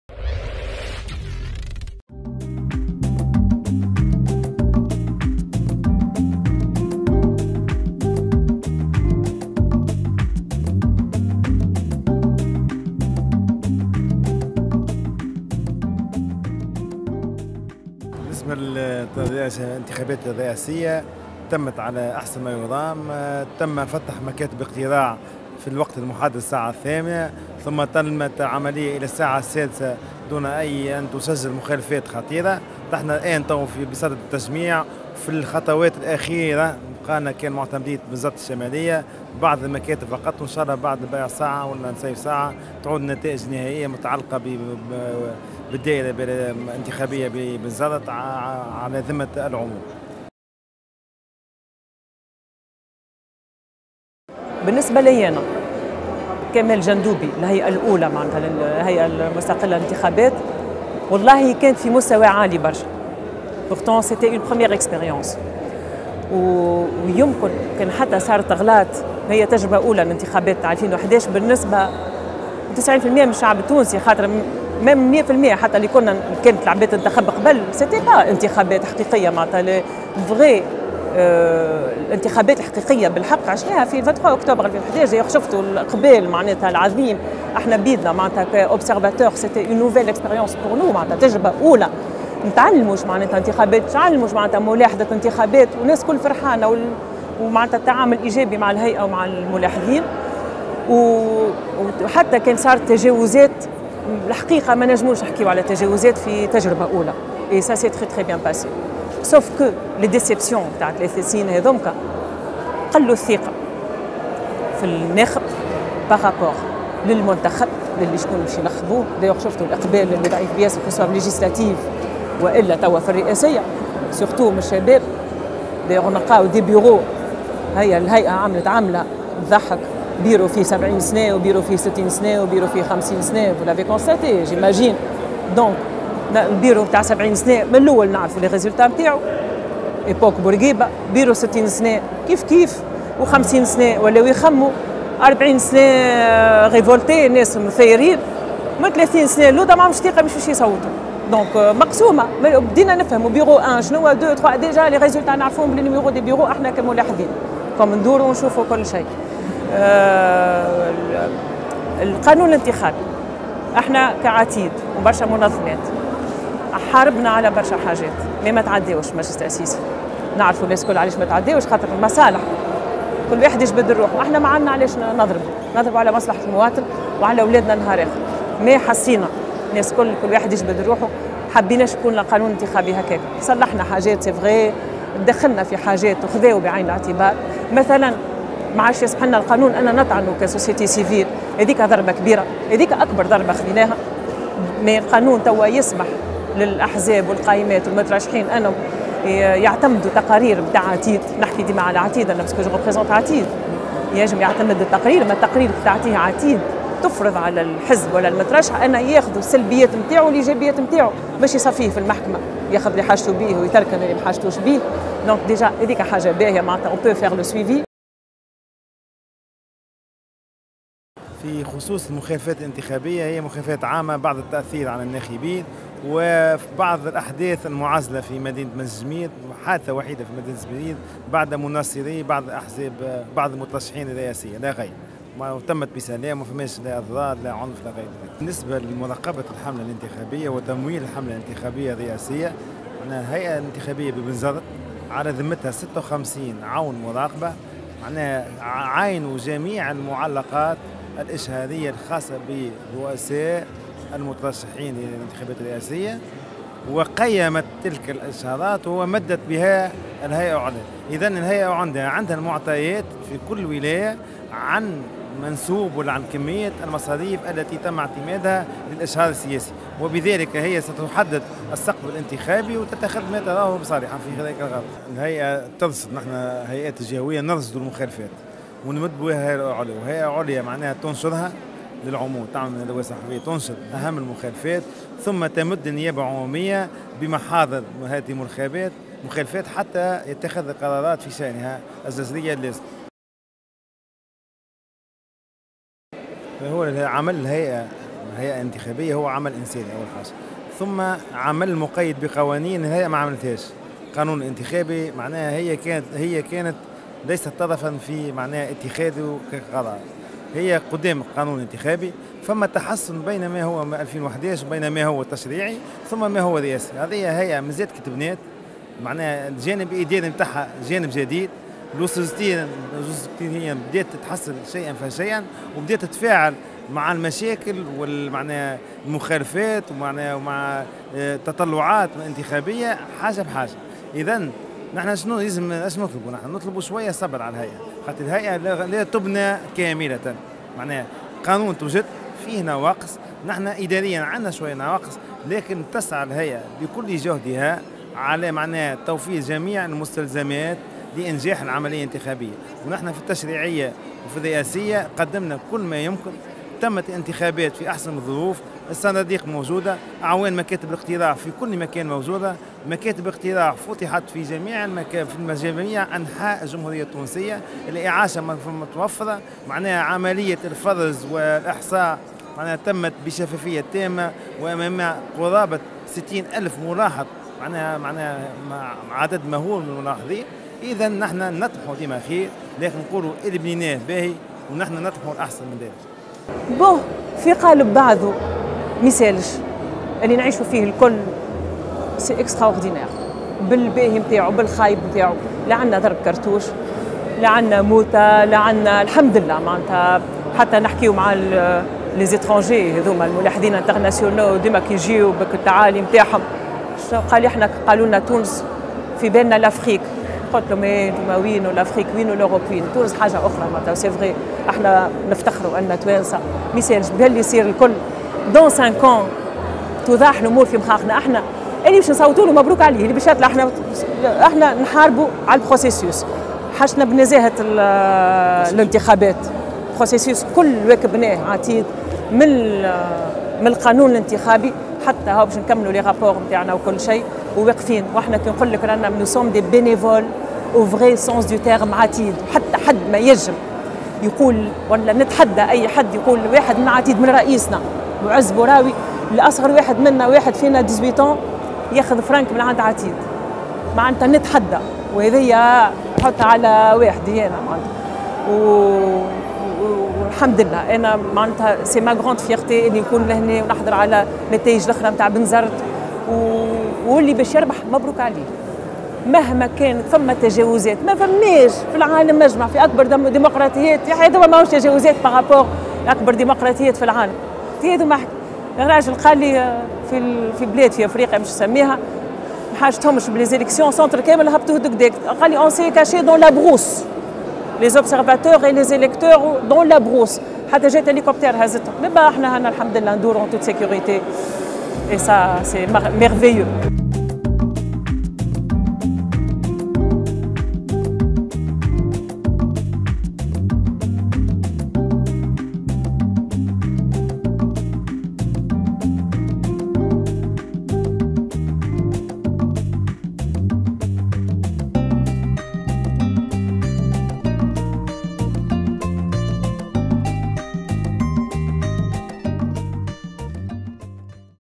Regards croisés sur le processus électoral dans la circonscription de Bizerte
Pour le 24 novembre 2014, la caméra de Nawaat s’est déplacée au centre de tri de la circonscription électorale de Bizerte. C’est l’une des plus importantes circonscriptions de Tunisie, tant par sa superficie que par le nombre de ses électeurs.

Nous avons choisi de croiser les regards de deux personnalités de cette circonscription qui se sont investies dans le processus électoral de la région depuis le 23 octobre 2011.

Pour la première personnalité, il s’agit de Sihem Zaouali, l’une des figures de la ville de Bizerte, militante de longue date au sein de la société civile de la région. Elle a suivi, en tant qu’observatrice avec ATIDE, les différentes élections depuis le 23 octobre 2011.

Il en fut de même de Maître Goutali, membre de l’instance régionale des élections de la circonscription de Bizerte, qui participa, déjà, à l’organisation des premières élections libres de Tunisie, en l’occurrence celle de l’Assemblée nationale constituante. C’était la fin d’une longue journée, et malgré la fatigue accumulée depuis des jours, Maître Goutali a bien voulu consacrer quelques minutes pour répondre aux questions de Nawaat.

Nous vous laissons, au sein de la présente vidéo, avec ces regards croisés sur le processus électoral dans la circonscription de Bizerte…